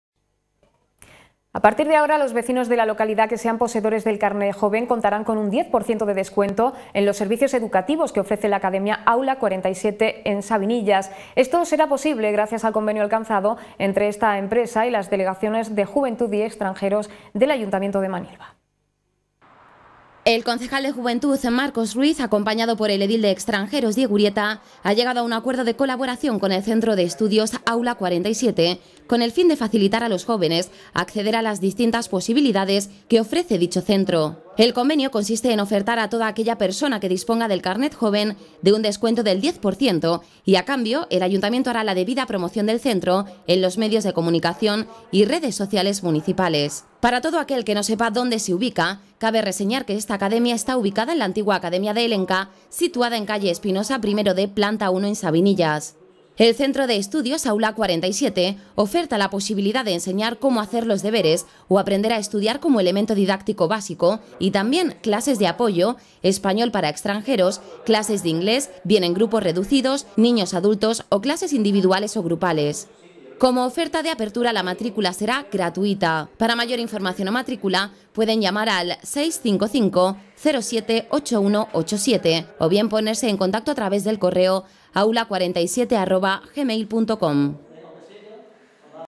CONVENIO DE COLABORACIÓN CON AULA 47.
El concejal de Juventud, Marcos Ruiz, acompañado por el edil de Extranjeros, Diego Urieta,  ha llegado a un acuerdo de colaboración con el Centro de Estudios, Aula 47,  con el fin de facilitar a los jóvenes acceder a los distintas posibilidades que ofrece dicho centro.

El convenido consiste en ofertar a todo aquella persona que disponga del carnet joven, de un descuento del 10%,  y a cambio, el Ayuntamiento hará la debida promoción del centro,  en los medios de comunicación y redes sociales municipales.

Para todo aquel que no sepa dónde se ubica, cabe reseñar que esta academia esta ubicada en la antigua academia de Helenka, situada en C/Espinosa, 1D-Planta 1 en Sabinillas.

El Centro de Estudios Aula 47 oferta la posibilidad de enseñar cómo hacer los deberes o aprender a estudiar como elemento didáctico básico y también clases de apoyo, español para extranjeros, clases de inglés bien en grupos reducidos, niños/adultos o clases individuales o grupales.

Como oferta de apertura, la matrícula será gratuita.

Para mayor información o matrícula pueden llamar al o bien ponerse en contacto a través del correo